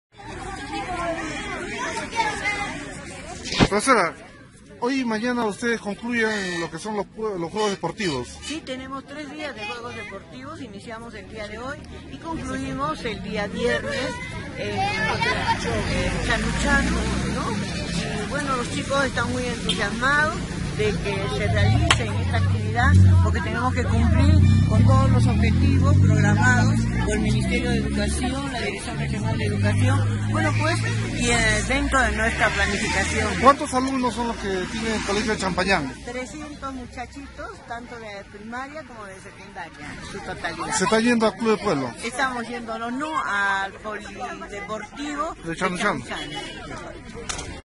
Niños de Puno participan en los Juegos Deportivos Escolares
Alumnos del Colegio Champagnat del Niño Divino Jesús hoy participan en los Juegos Escolares 2017, cerrando el certamen de este año. Ellos a pié y en bicicletas fueron hasta la Plataforma Deportiva de Chanu Chanu donde los demás colegios hicieron lo propio.